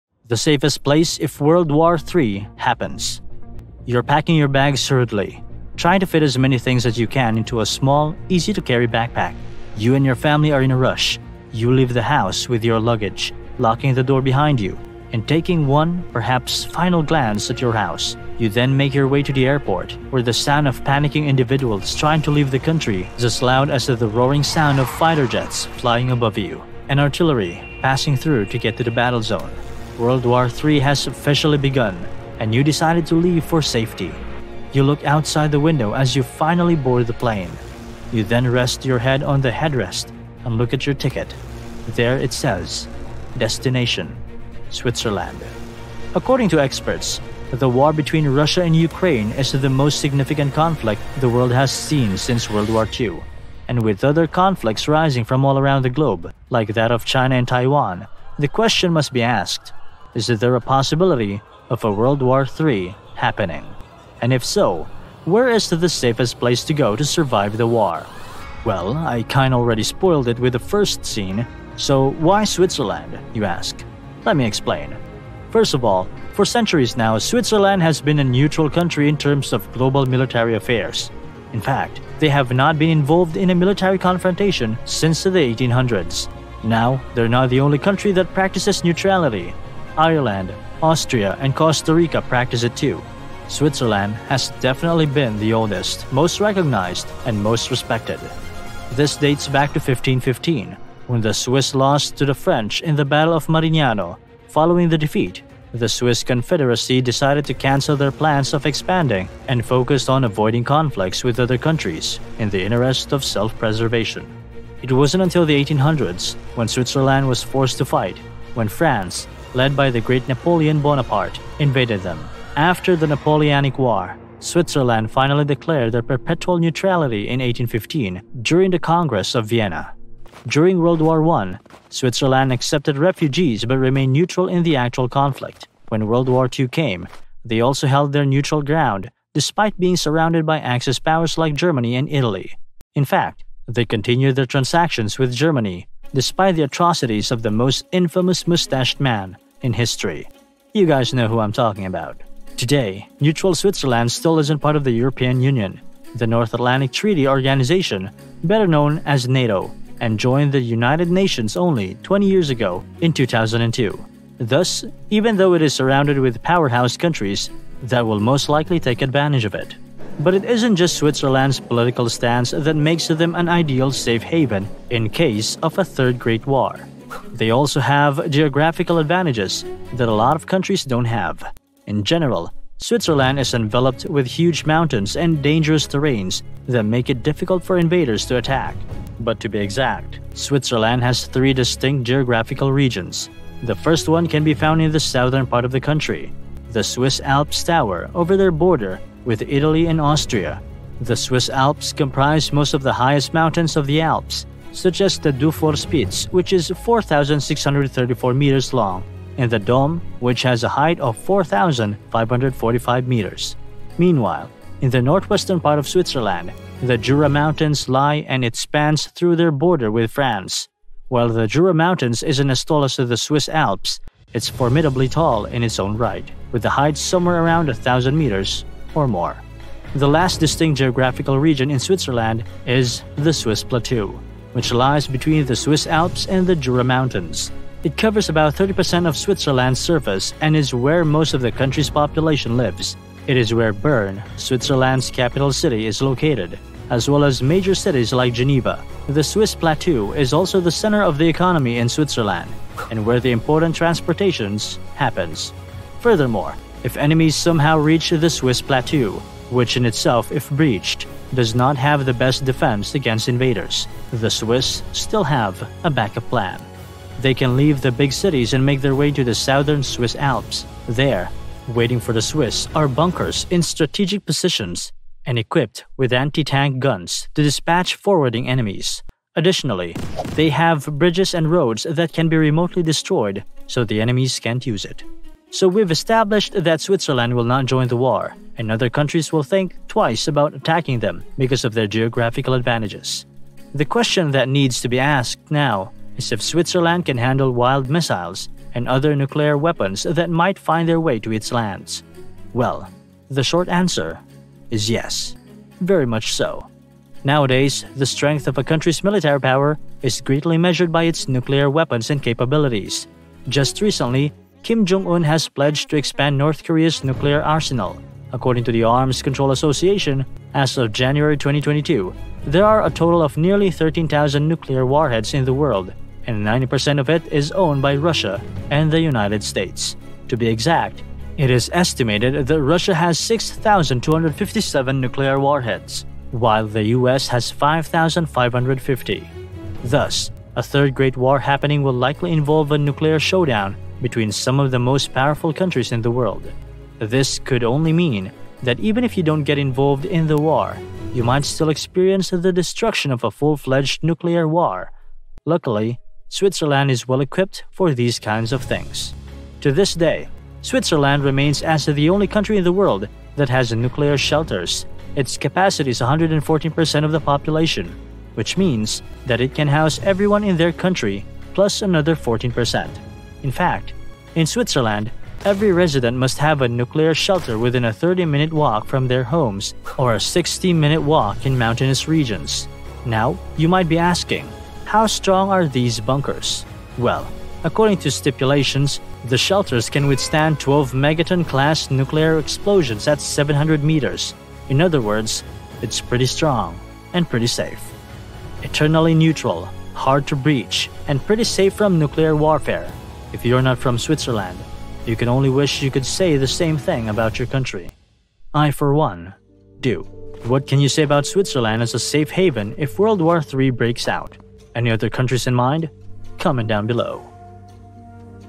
The Safest Place If World War 3 Happens
Here's why Switzerland is the safest place if world war 3 happens.